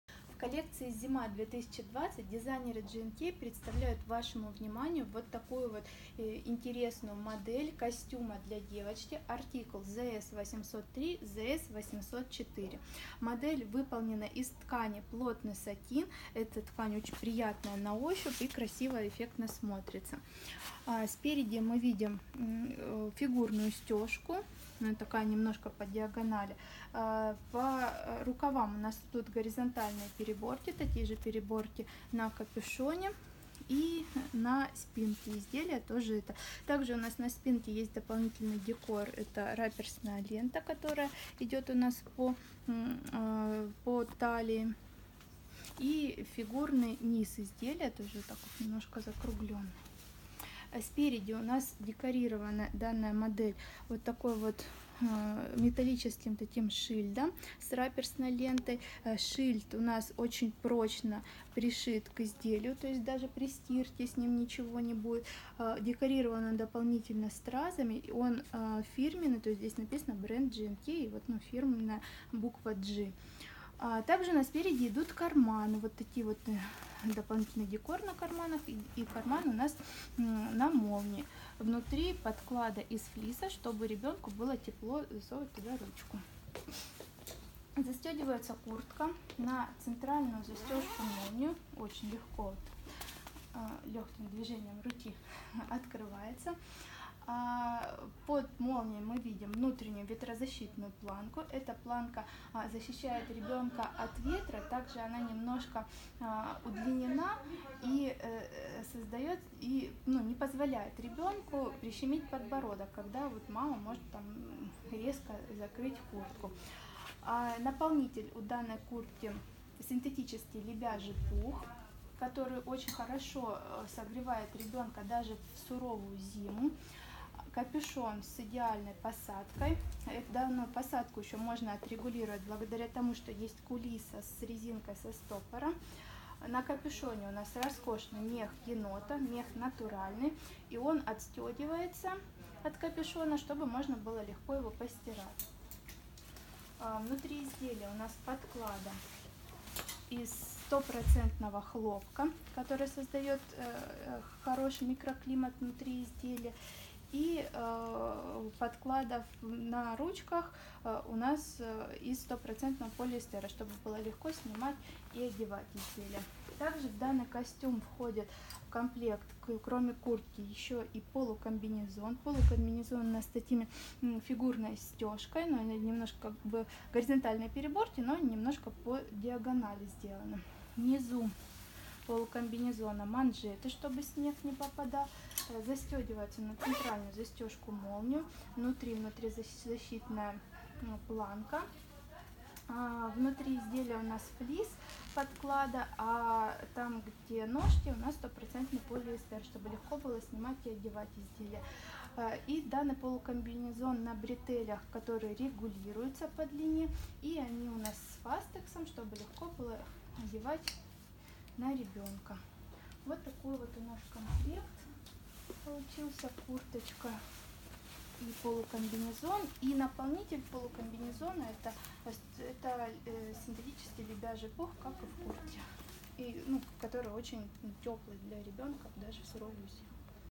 ЗС-803/ЗС-804 ОБЗОР
Артикул: ЗС-803/ЗС-804
Размеры: 110, 116, 122, 128 см
Ткань: плотный сатин
Наполнитель: куртка (наполнитель - синтетический лебяжий пух), полукомбинезон (наполнитель - синтетический лебяжий пух)
Подклада: куртка - 100% хлопок, полукомбинезон - 100% полиэстер
Опушка: мех енота натуральный

Декор: Застежка-молния-декоративная со звеном спираль серебро+ курсоры со стразами - центральная застежка, карманы. Бант из атласной и раперсной декоративной ленты + дизайнерский шильд со стразами из чешского богемского стекла- левый перед.

Описание: Костюм состоит из куртки и полукомбинезона. Куртка-прямого округлого силуэта. Рукав втачной, со слегка спущеной линией плеча. Центральная застежка на открытую тесьму-молнию, с усиленной нижней ветрозащитной планкой. Воротник-стойка. Перед куртки и нижняя часть спинки-стеганые фигурной стежкой, верхняя часть спинки и рукав-горизонтальными переборками. Спинка с кулисой по линии талии и раперсной тесьмой для регулировки. Низ изделия фигурный, с удлиненной спинкой-подол годэ.На переде выполнены прорезные карманы с тесьмой молнией и декоративными клапанами. Внутренняя часть воротника и подкладки карманов выполнены из флиса. С внутренней стороны, низ рукава, обработан трикотажным напульсником. Капюшон втачной, несъемный. По краю капюшона обработана регулируемая кулиса с резинкой и стопорами. Перед полукомбинезона стеган фигурной стежкой. Боковой шов смещен в сторону переда. Бретели-на резинке с фастексом, регулируемые по длине. Верхняя часть полукомбинезона застегивается на молнию с нижней ветрозащитной планкой. Пояс на спинке с резинкой для удобства прилегания. Низ полукомбинезона с притачными манжетами, которые при необходимости можно использовать для регулировки длины. С внутренней стороны низ обработан снегозащитной муфтой на резинке с латексом.